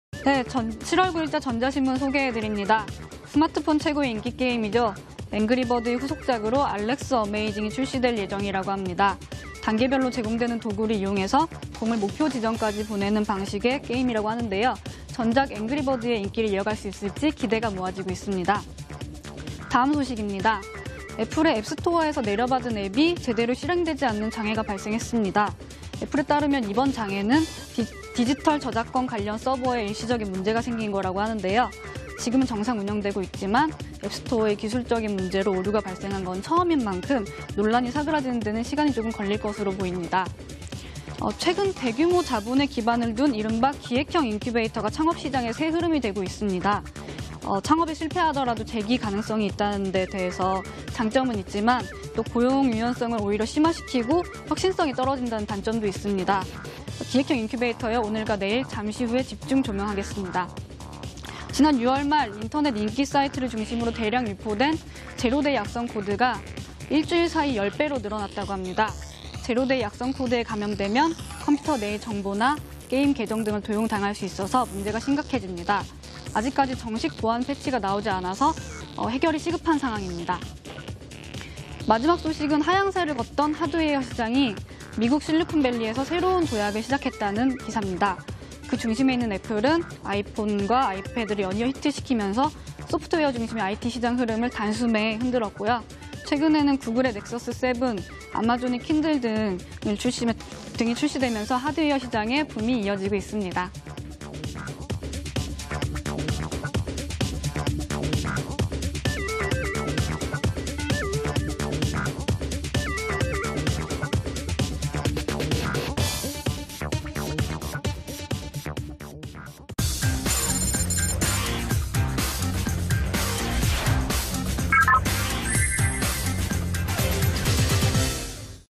[IT뉴스_2012.07.09] 앵그리버드의 후속작 '알렉스 어메이징'출시예정
앵그리버드의 후속작 '알렉스 어메이징'출시예정 / 앱스토어 앱플레이 장애 / 제로데이악성코드 일주일 새 10배 증가